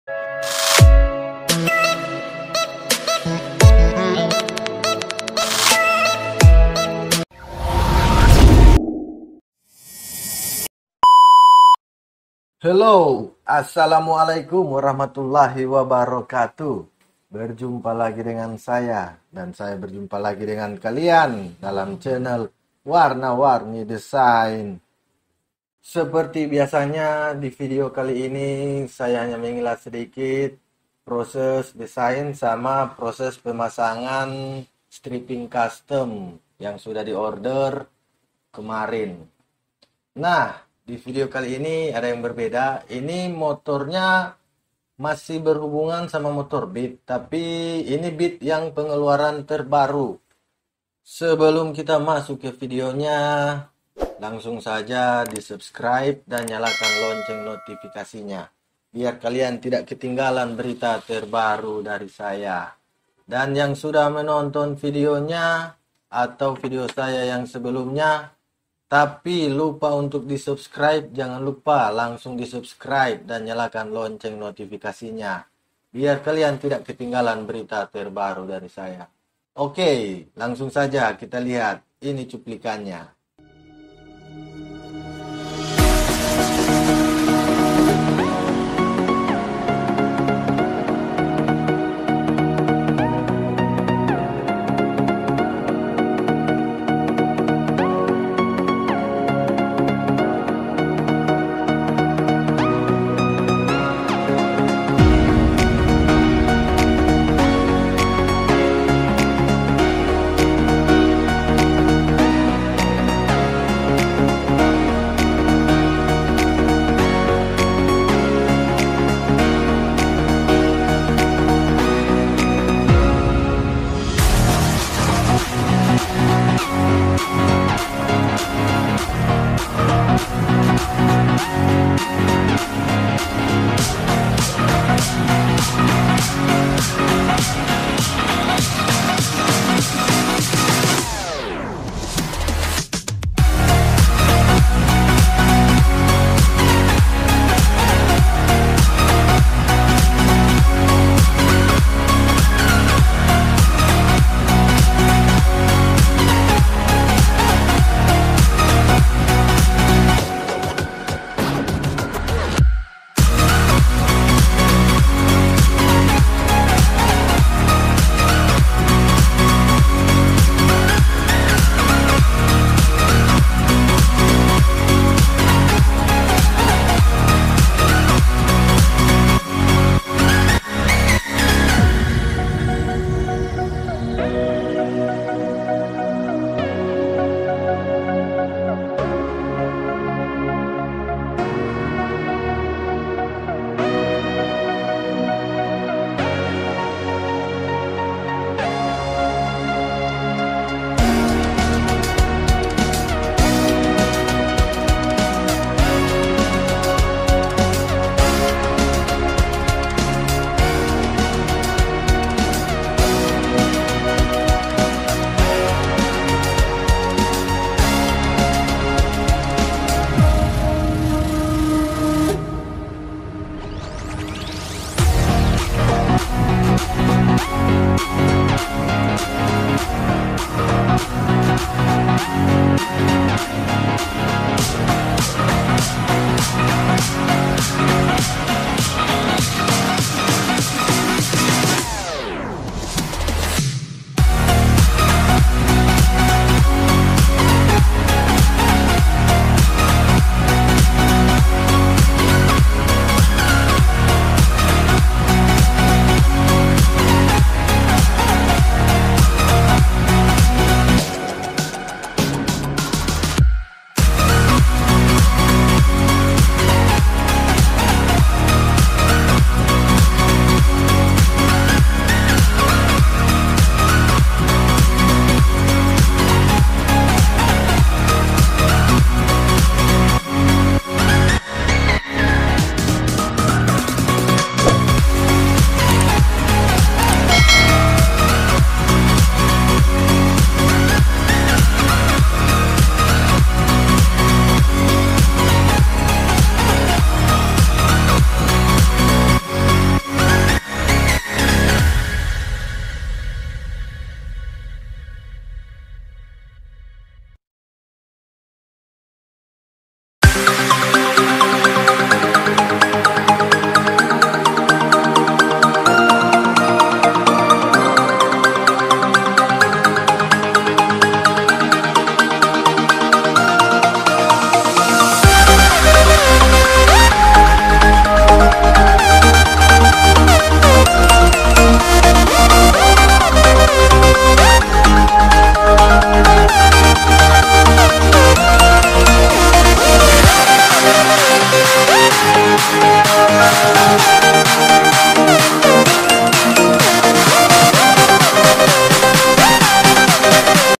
waauuu.... desain stiker+ pasang stiker striping motif road di beat terbaru.
kali ini saya dapat orderan untuk membuat sticker striping di motor beat terbaru
info orderan langsung saja DM di insta gram saya, link nya ada di bawah ini

desain sticker striping
desain sticker decal
desain vector
desain graphich
pemasangan sticker striping/decal

wassalamu'alaikum